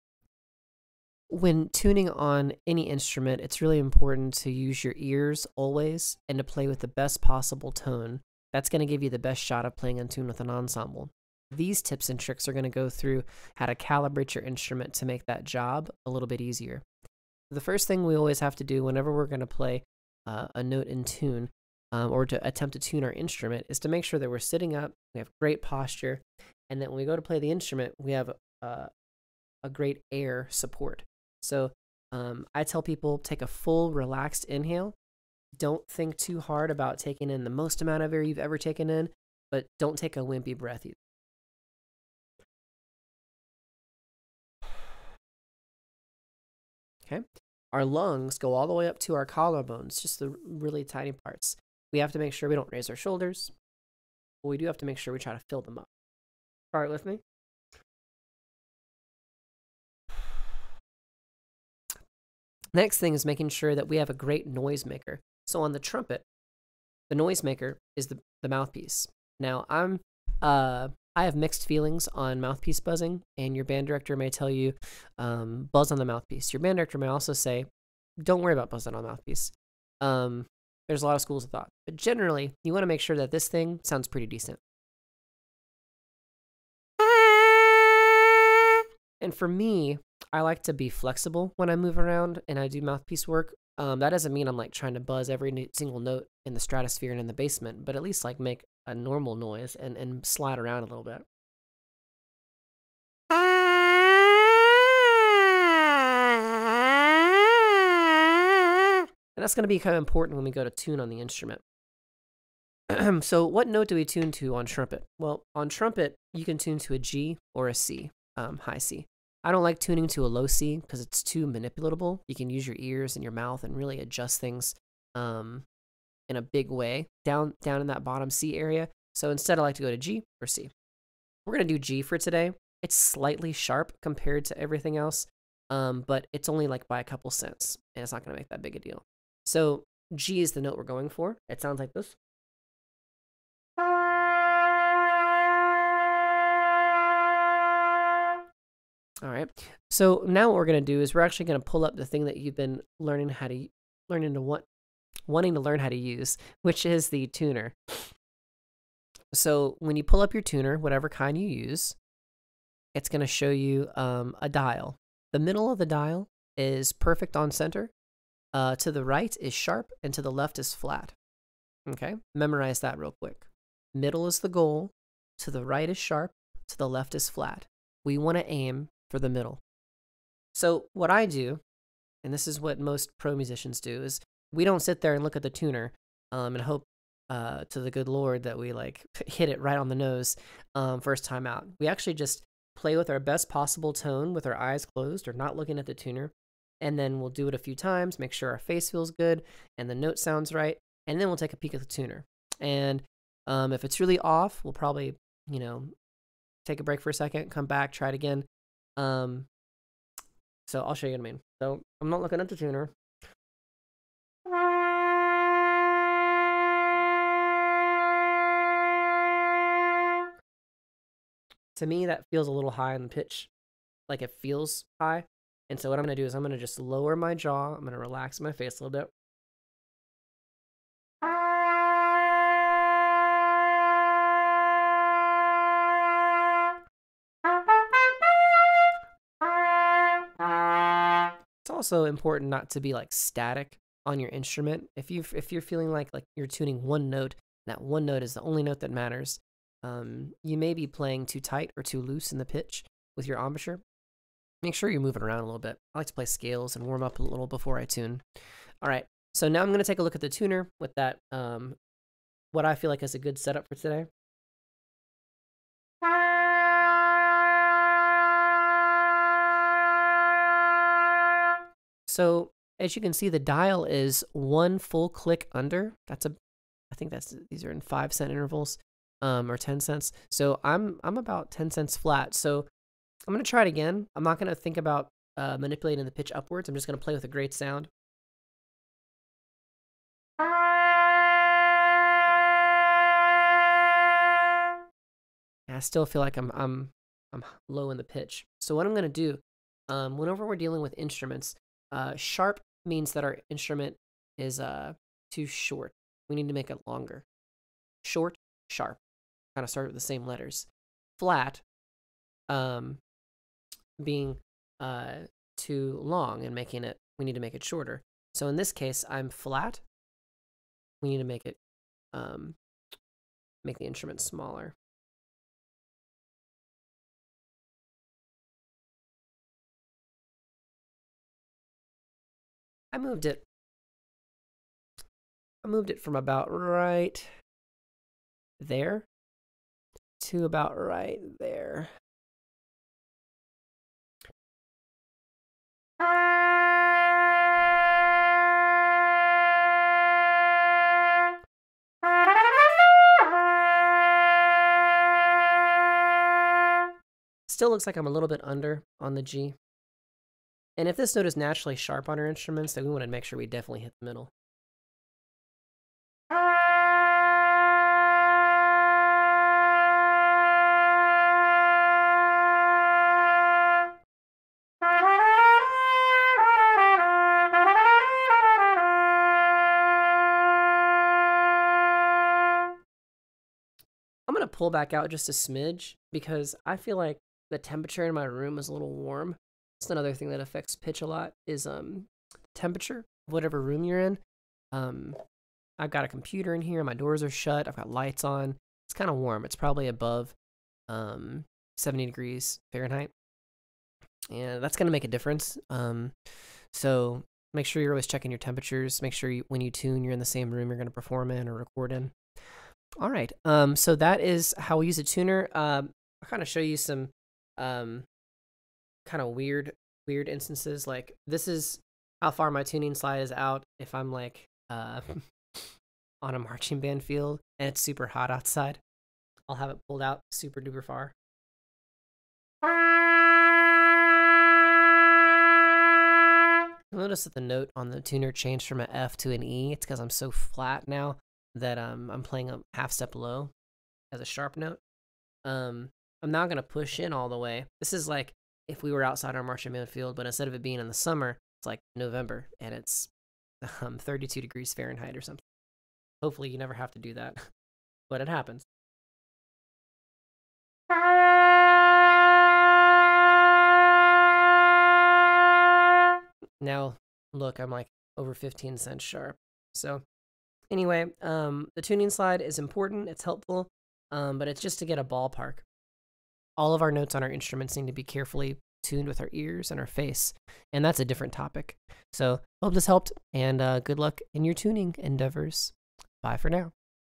How to tune the trumpet
1:05-Noise maker
2:05-What note to tune to
2:35-The tuner and how to use it
3:02-Best practices with the tuner
6:30-Tempeture and how it affects tuning